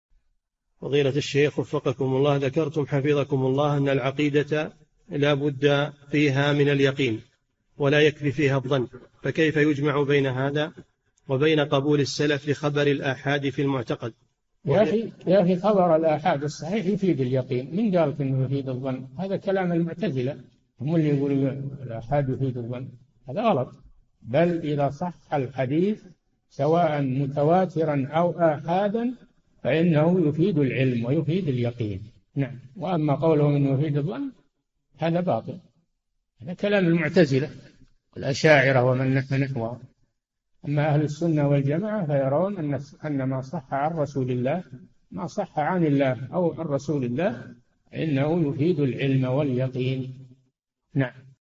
[2808- 4717] هل قبول السلف لخبر الآحاد في العقيدة ينافي القول بأن العقيدة لا تبنى على أدلة ظنية؟
[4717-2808] ذكرتم حفظكم الله أن العقيدة لابد فيها من اليقين ولا يكفي فيها الظن فكيف يجمع بين هذا وبين قبول السلف لخبر الآحاد في المعتقد؟
الجَوَابُ: يا أخي، خبر الآحاد الصحيح يفيد اليقين من قال لك أنه يفيد الظن؟ هذا كلام المعتزلة هم الذين يقولون الآحاد يفيد الظن هذا غلط بل إذا صح الحديث سواء متواترا أو آحادا فإنه يفيد العلم ويفيد اليقين وأما قولهم إنه يفيد الظن هذا باطل هذا كلام المعتزلة الاشاعرة ومن نحا نحوهم أما أهل السنة والجماعة فيرون أن ما صح عن الله أو عن رسوله فإنه يفيد العلم واليقين.
السيرة الذاتية :
للاشتراك في قناة الشيخ صالح الفوزان
لمشاهدة حلقات فتاوى على الهواء كاملة

للاستفسار والإقتراحات والدعم
رئيس الفريق
لمشروع كبار العلماء